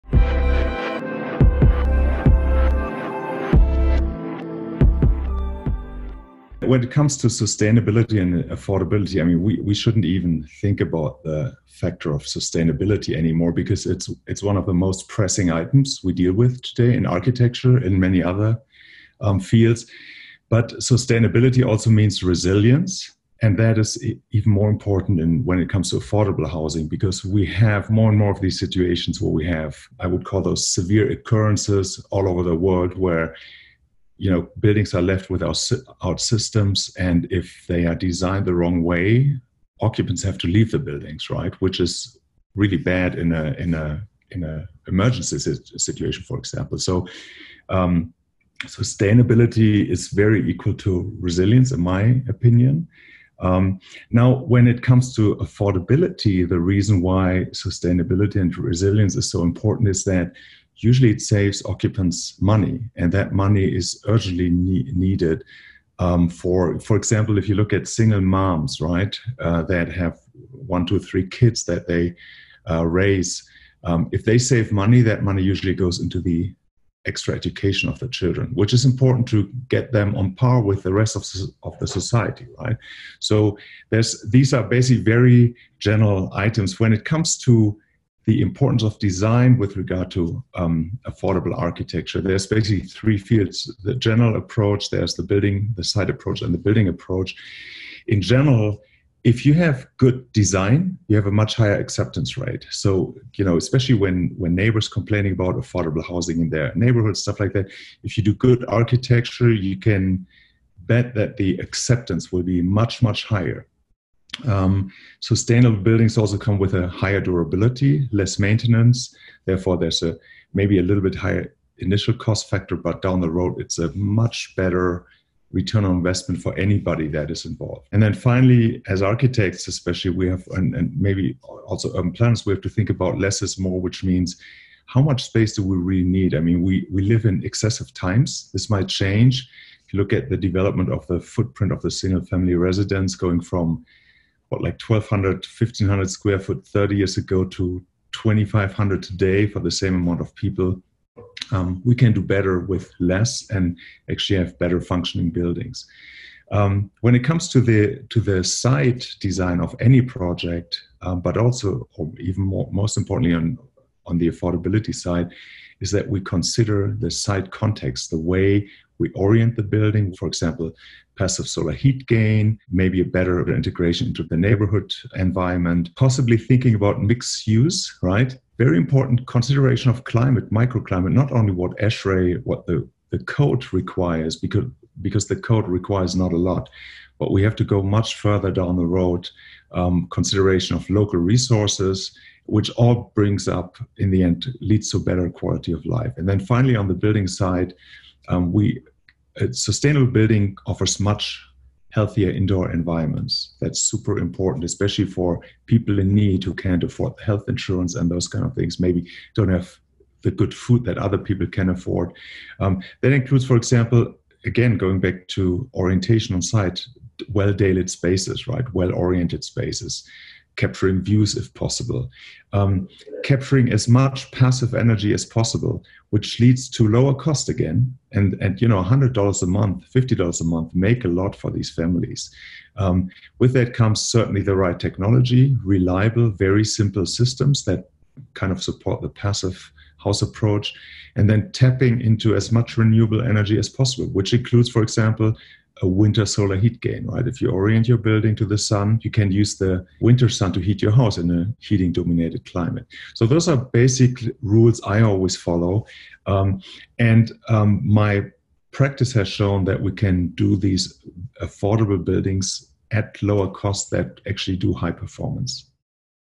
Jorg Rugemer Hack-A-House Thoughts
Jorg Rugemer - Director, Design+Build Salt Lake Program, Associate Professor, Utah College of Architecture, Architect, AIA - discusses his thoughts on sustainable and affordable design. Teaching, Research, Social Justice and Creative Practice all converge.